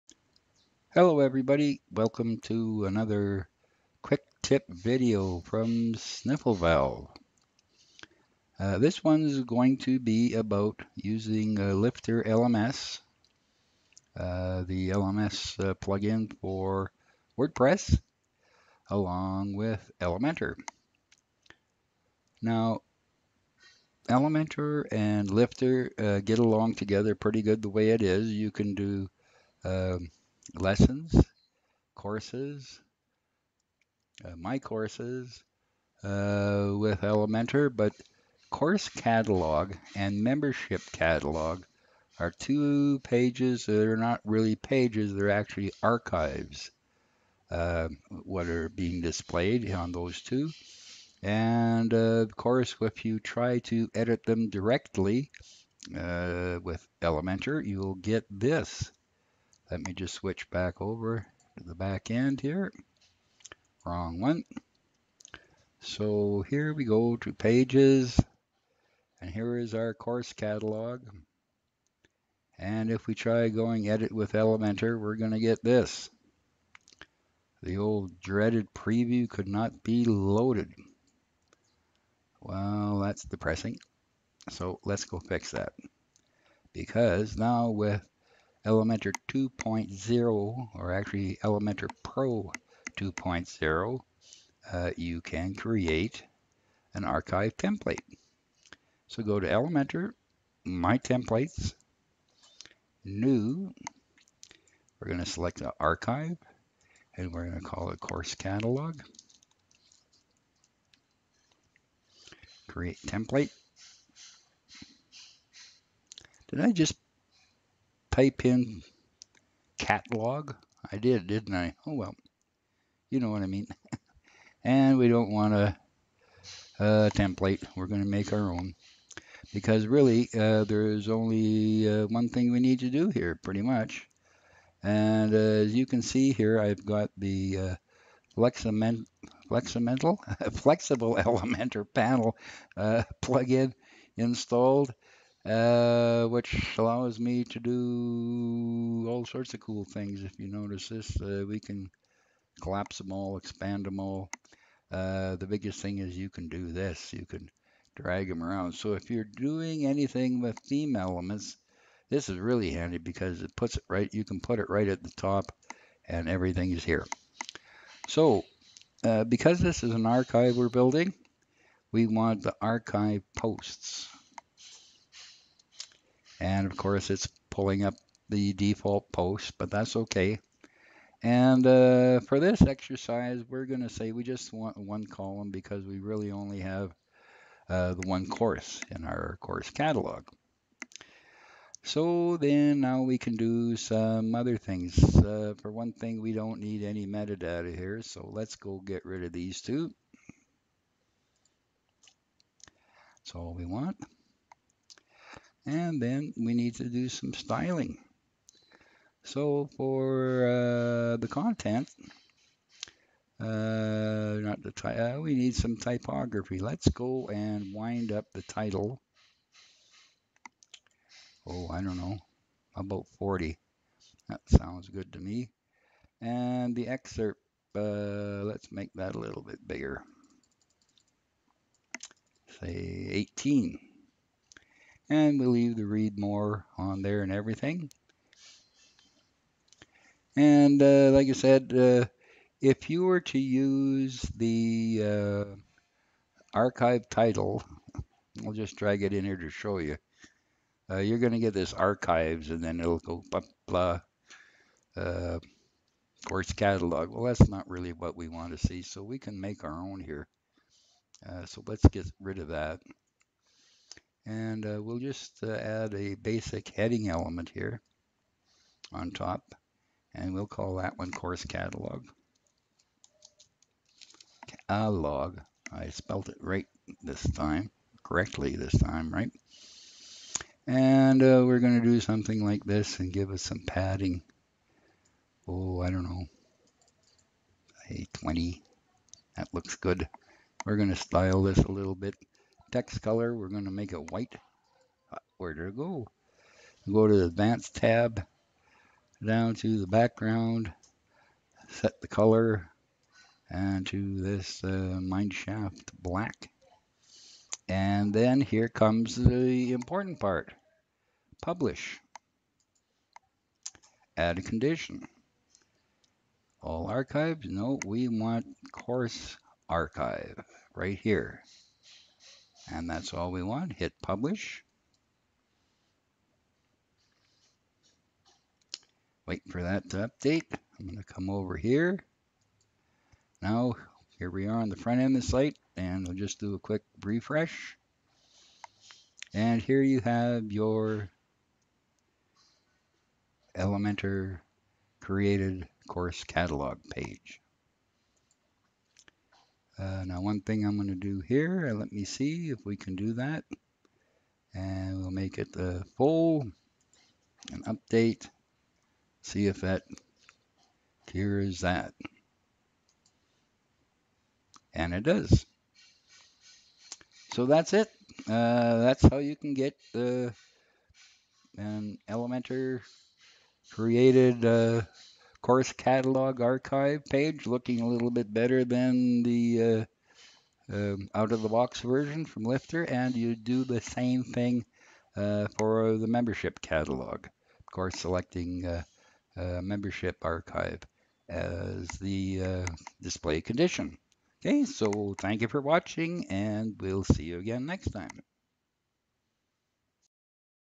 Elementor Pro - Create a LifterLMS Course Catalog Archive
How to use Elementor Pro's Theme Elements to create a LifterLMS Course Catalog archive.